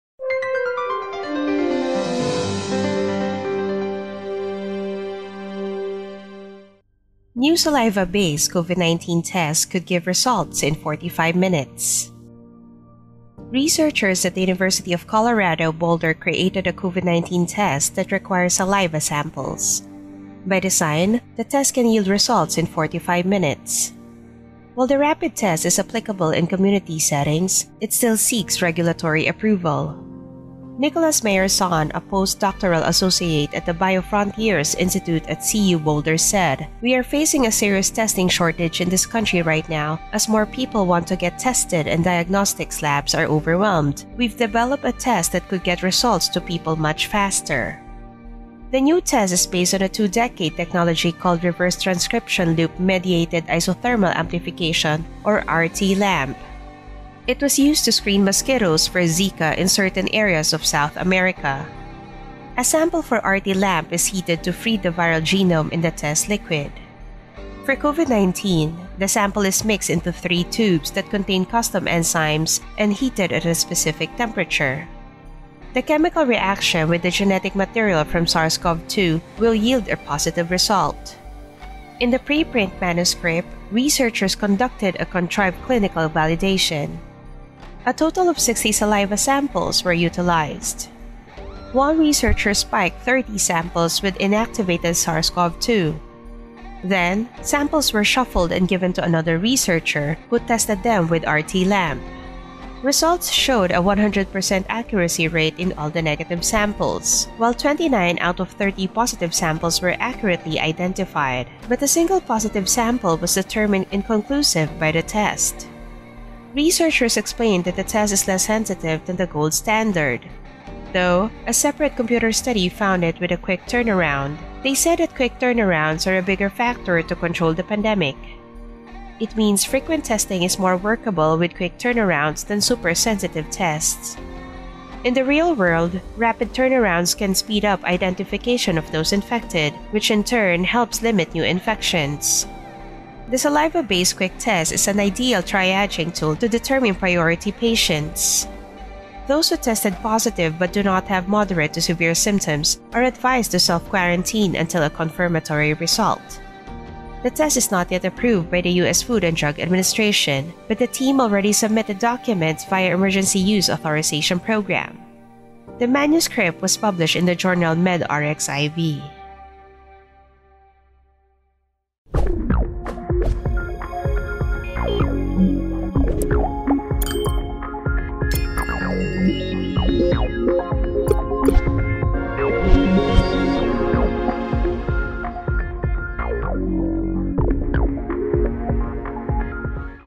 New Saliva Based COVID-19 Test Could Give Results in 45 Minutes
Researchers developed a new saliva-based test for COVID-19.